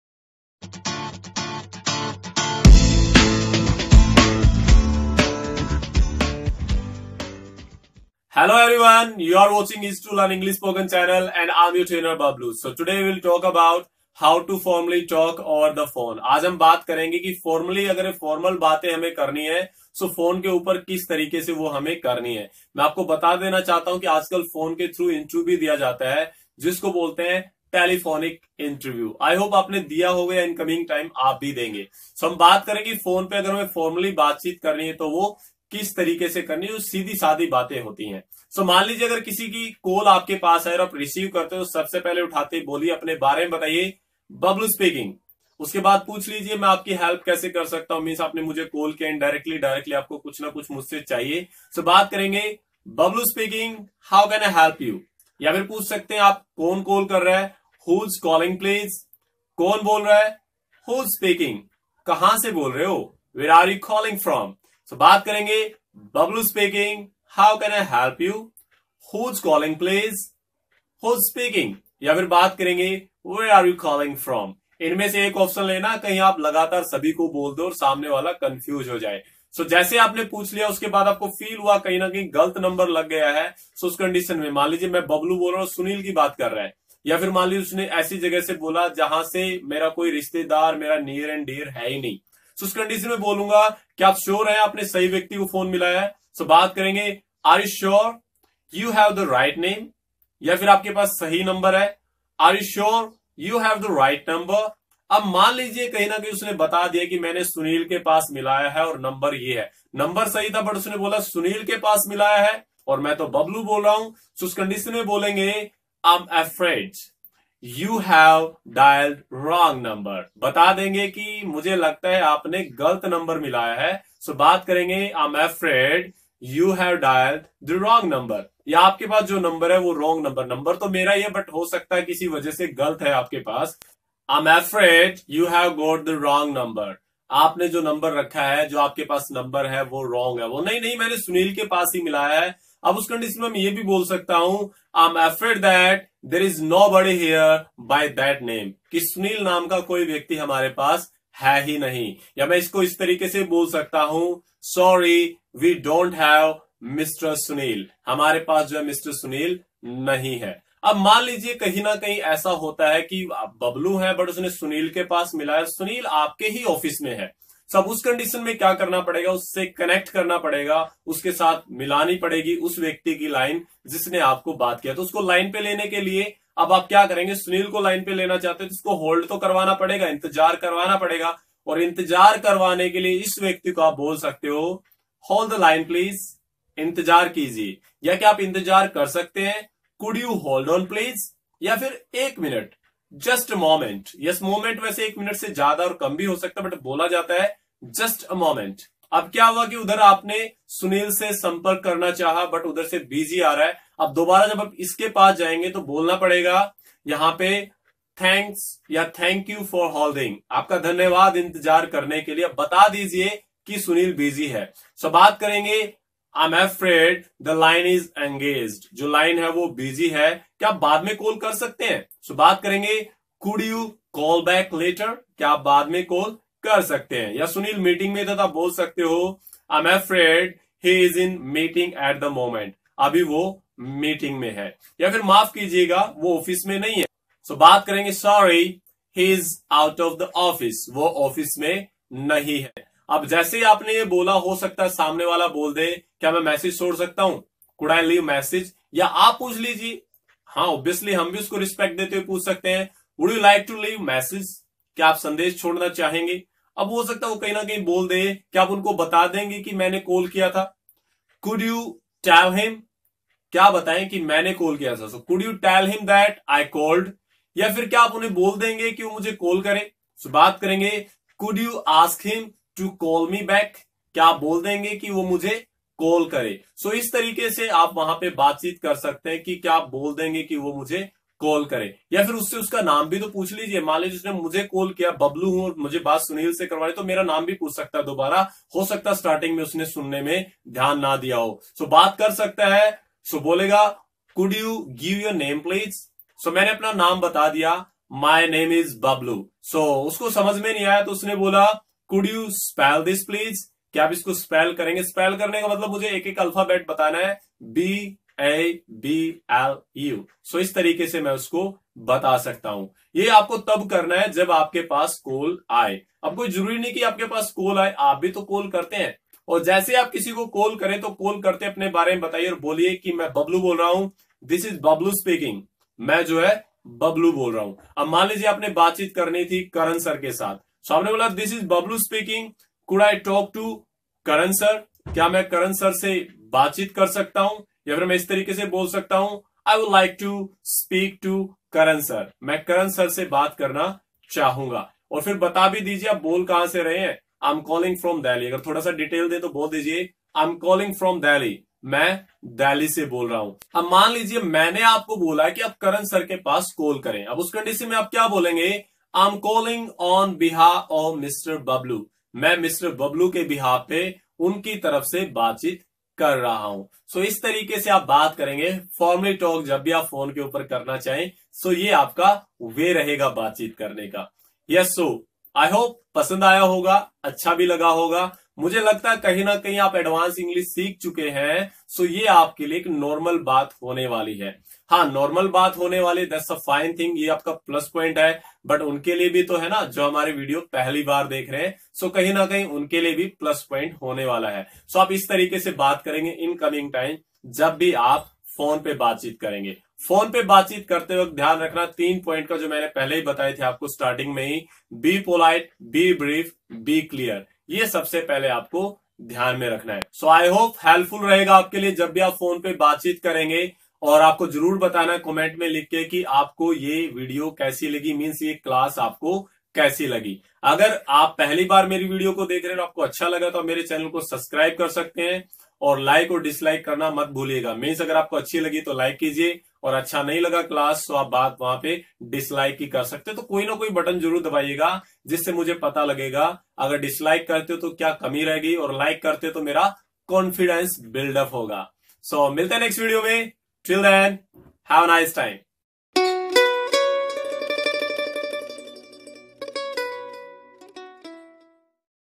Formally talk over the phone by Bablu Sir
Hello all
Today we we are gonna learn about
How to formally talk over the phone
So let's watch...